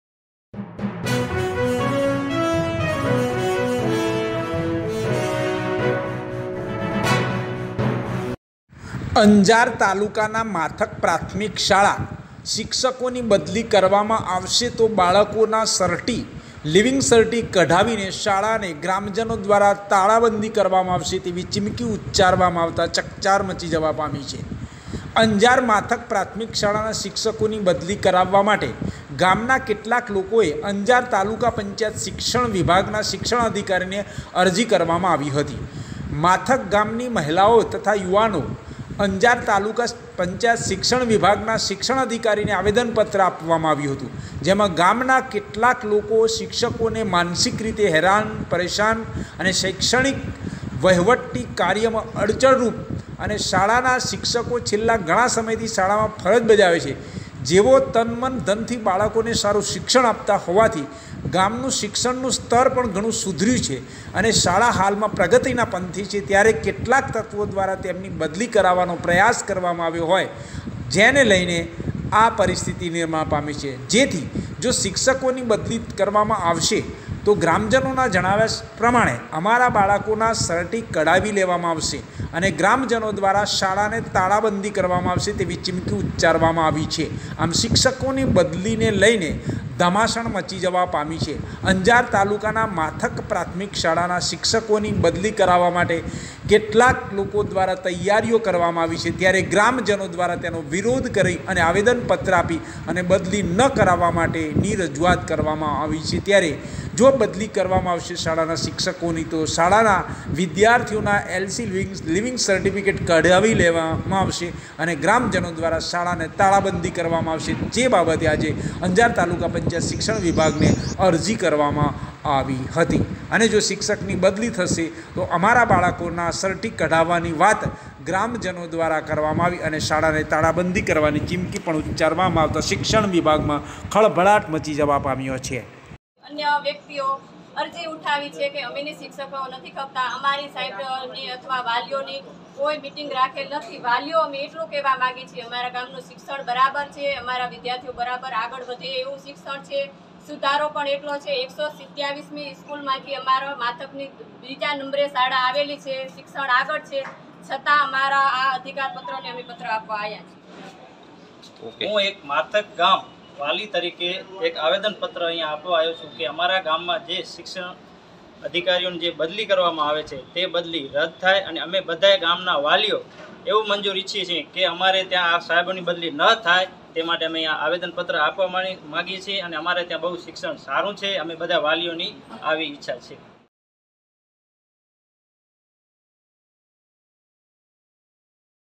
શિક્ષકોની બદલીઓ થશે તો ગ્રામજનો સર્ટી કઢાવી અંજાર માથક પ્રા. શાળાને તાળા બંઘીની ચીમકી ઉચ્ચારી
𝗟𝗮𝘁𝗲𝘀𝘁 𝗡𝗲𝘄𝘀